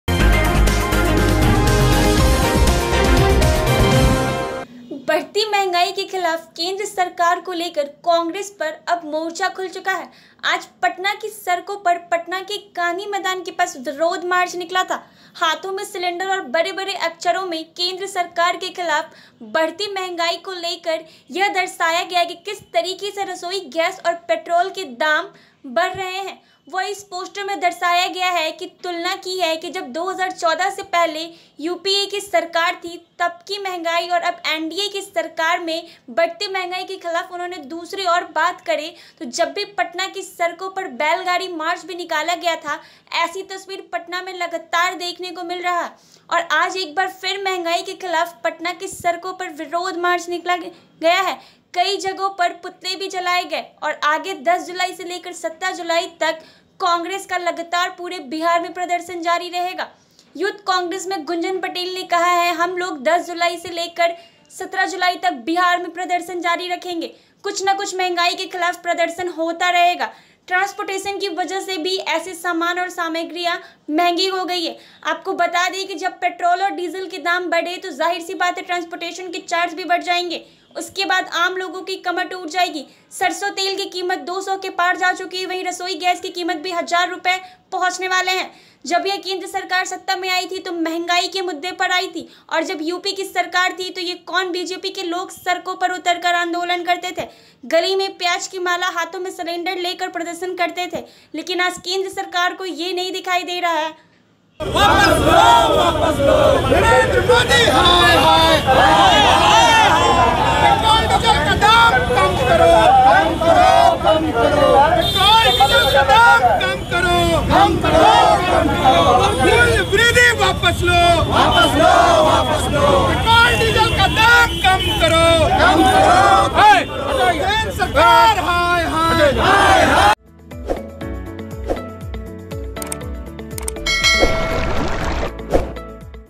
Patna के गांधी मैदान के पास बढ़ती महंगाई के खिलाफ केंद्र सरकार को लेकर कांग्रेस ने निकाला मोर्चा
About the Video - बढ़ती महंगाई के खिलाफ केंद्र सरकार को लेकर कांग्रेस अब मोर्चा खुल चुकी है. पटना की सड़कों पर गांधी मैदान के पास विरोध मार्च निकाला. हाथों में सिलेंडर और बड़े बड़े अक्षरों में केंद्र सरकार के खिलाफ बढ़ती महंगाई के खिलाफ पोस्टर में यह दर्शाया गया है कि किस तरीके से रसोई गैस और पेट्रोल के दाम में किस वृद्धि हुई है...